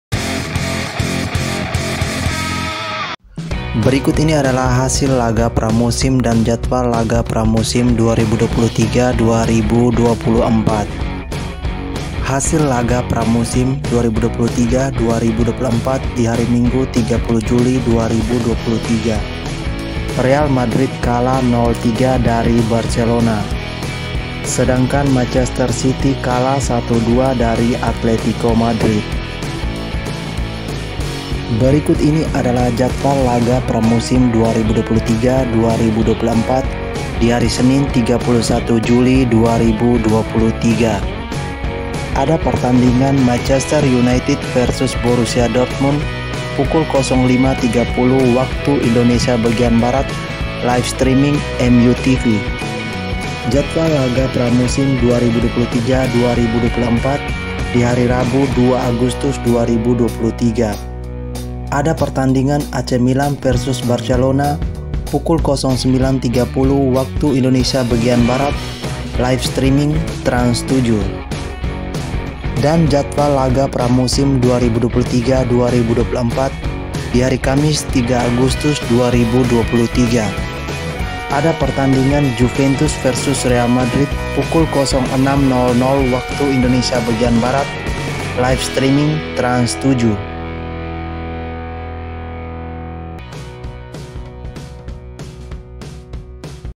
HASIL LAGA PRA~DAN JADWAL LAGA PRA - MUSIM - REAL MADRID VS BARCELONA-MANCESTER CITY VS ATLETICO
HASIL LAGA PRA~MUSIM  DAN  JADWAL LAGA PRA MUSIM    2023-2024